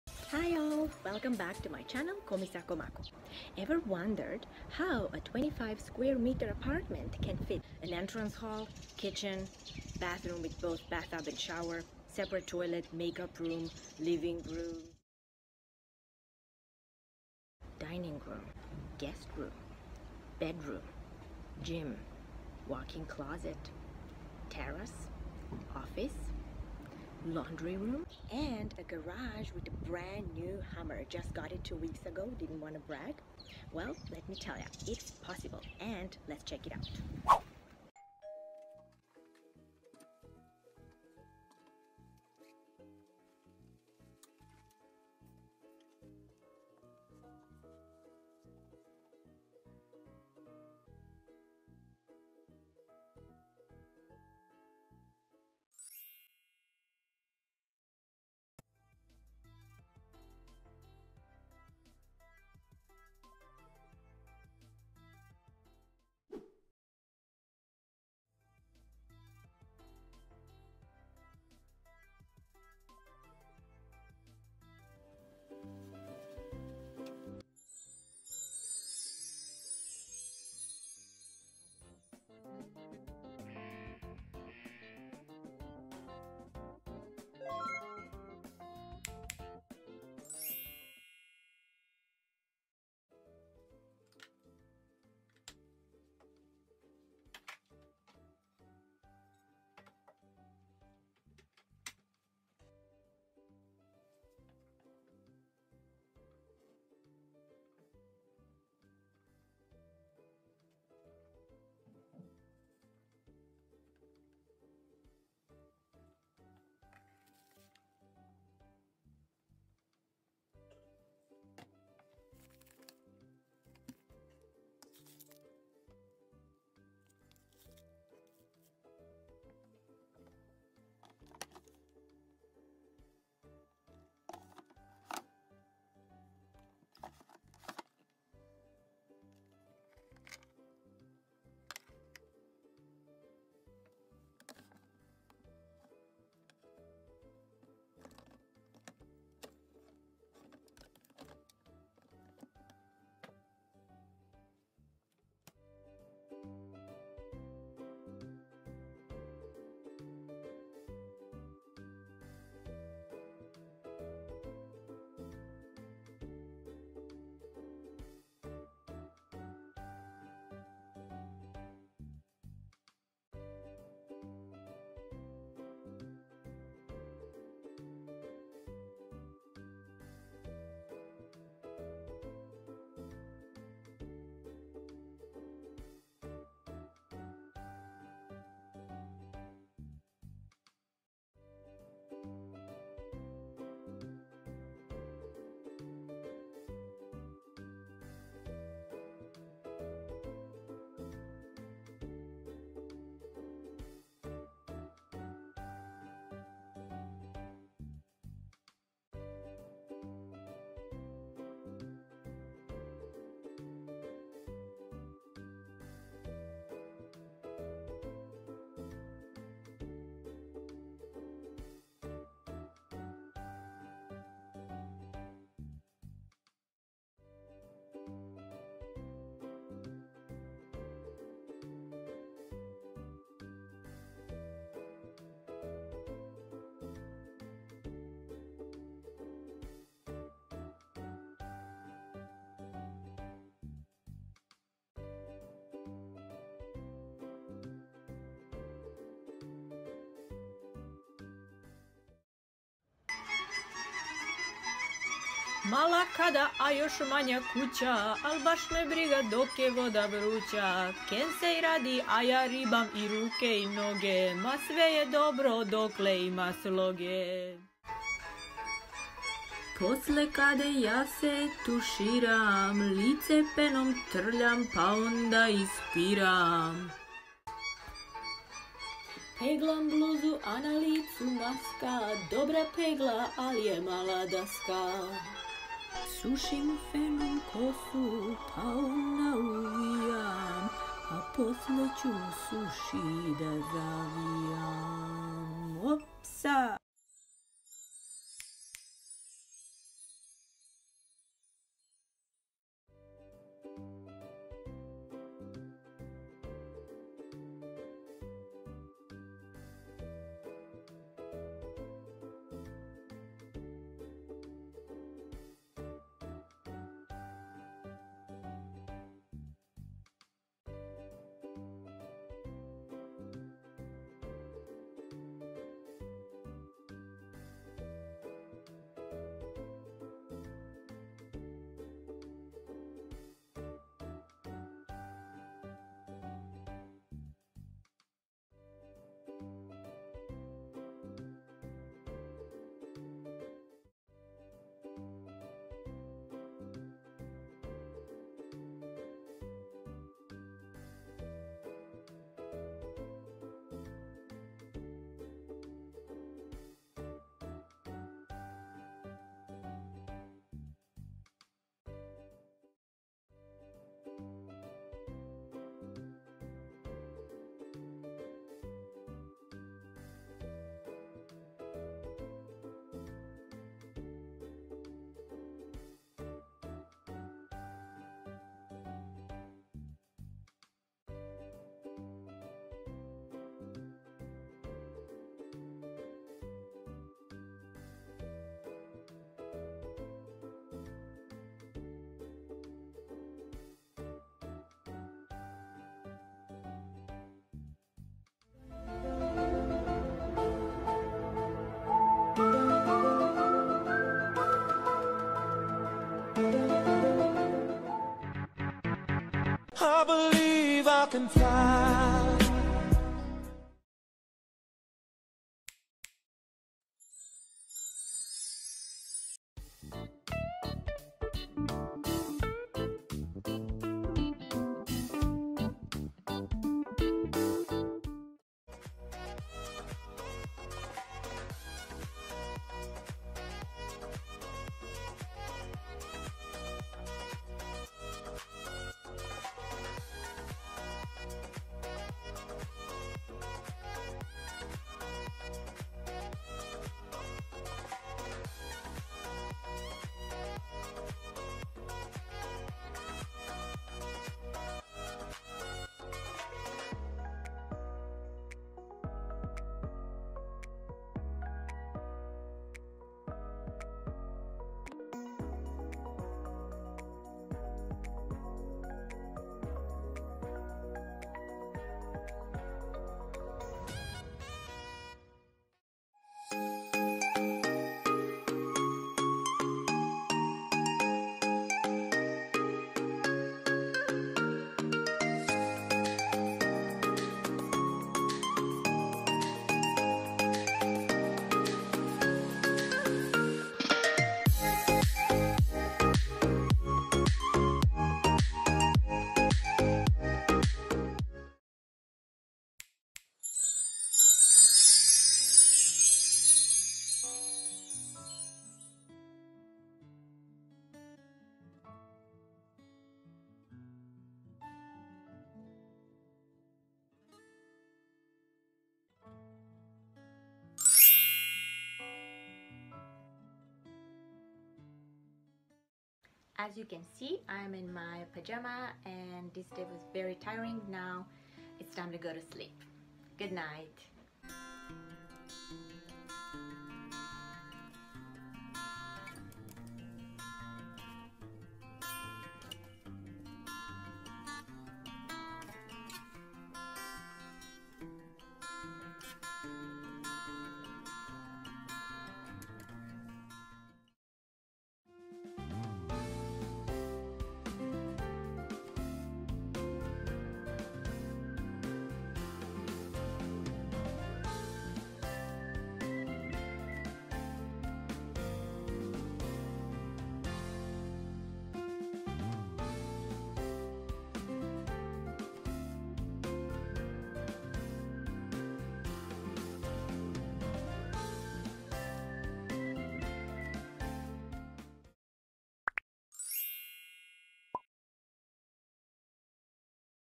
My Tiny Tokyo Apartment Tour | What to expect for 700 dollars a month ???
This video is a tour of my tiny Tokyo apartment.
My Japanese home is just 25m2 and it costs 700 dollars a month, but you'll see what to expect for that money and how it's possible to fit all you need in such a tiny space. I will show you my Japanese home organization, how to exercise in small space, where to take off shoes in Japan etc.

Musician: @iksonofficial

Musician: @iksonofficial